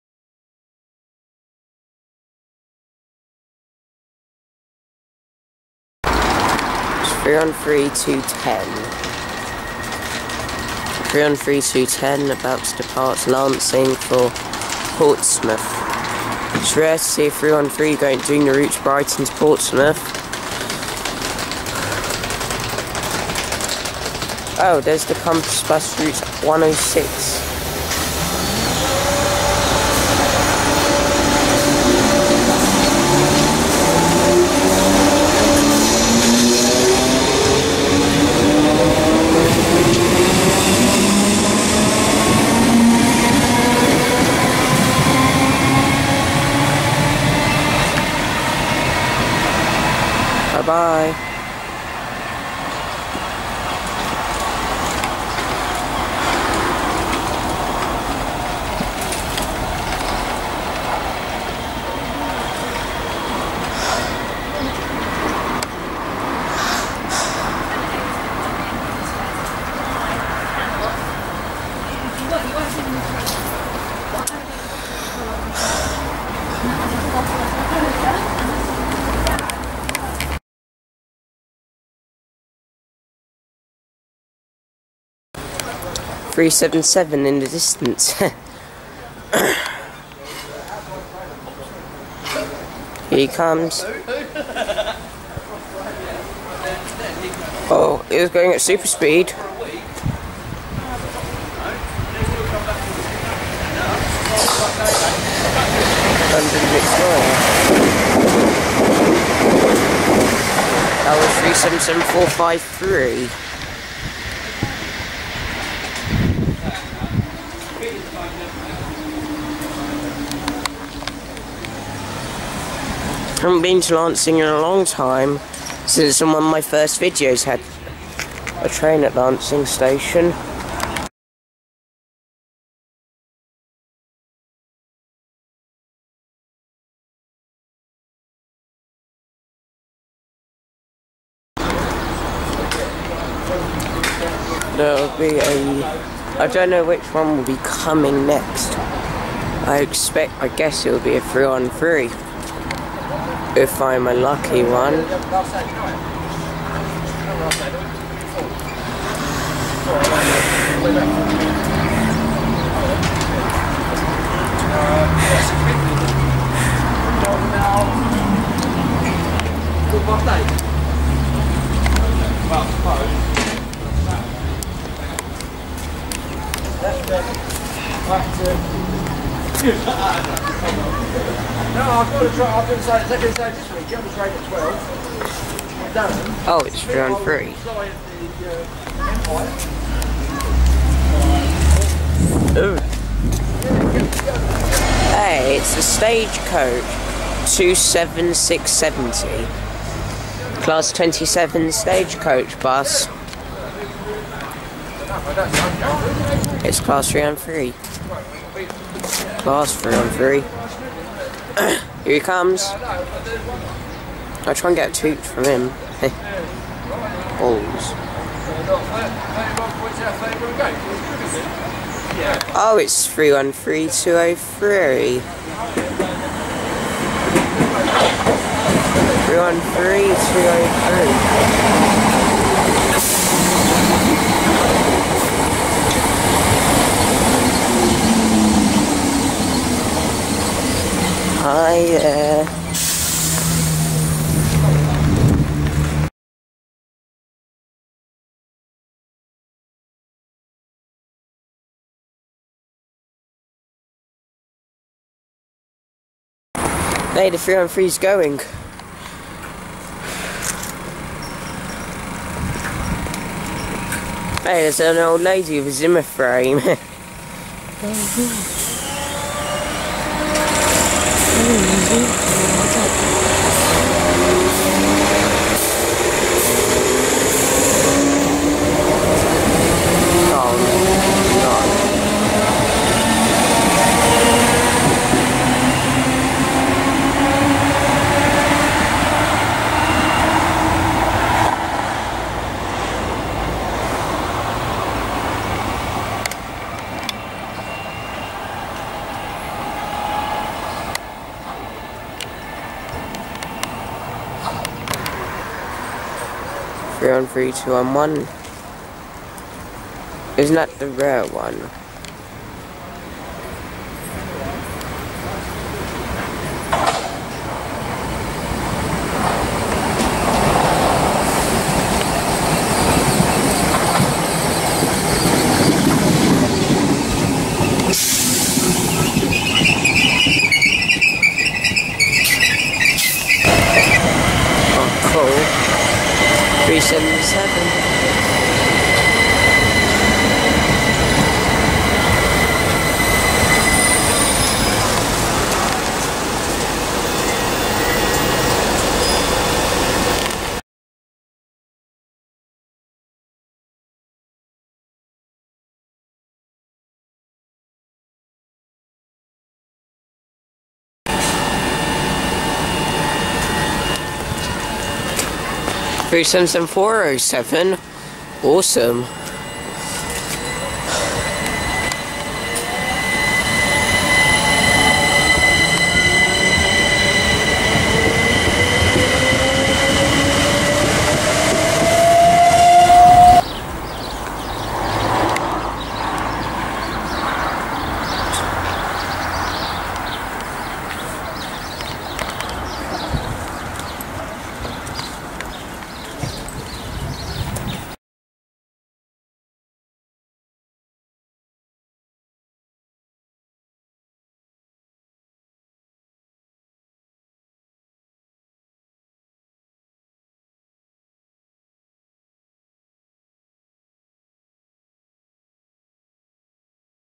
Trains at Lancing Station, 16th February 2015
Foxy Filming Trains at Lancing Station and its been 3 years since I last visited Lancing Station and It's a lovely and spectacular station :-)
Have a good evening Guys :D